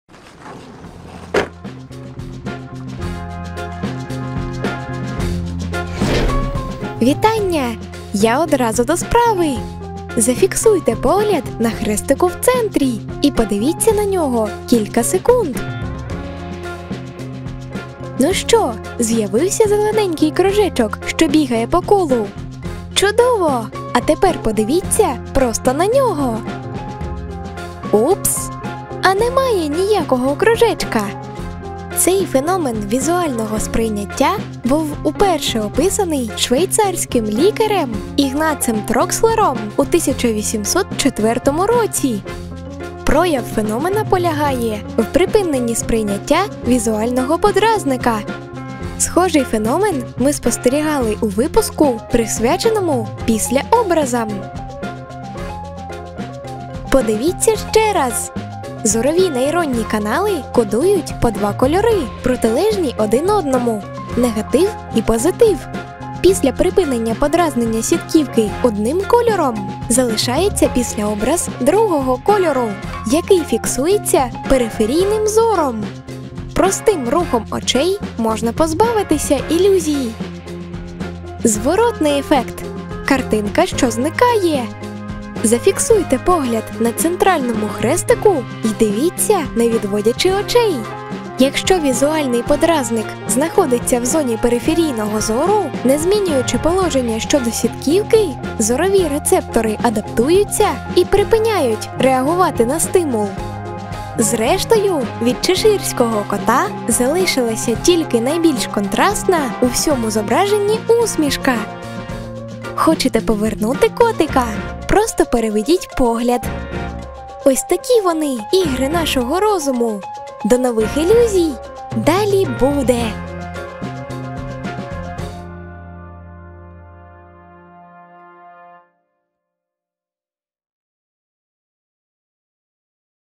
Оптичні ілюзії. Феномен Трокслера
Наша зорова система може бачити те, чого немає, і не бачити те, що просто під носом. Нове дослідження Уляни пояснює
цей феномен.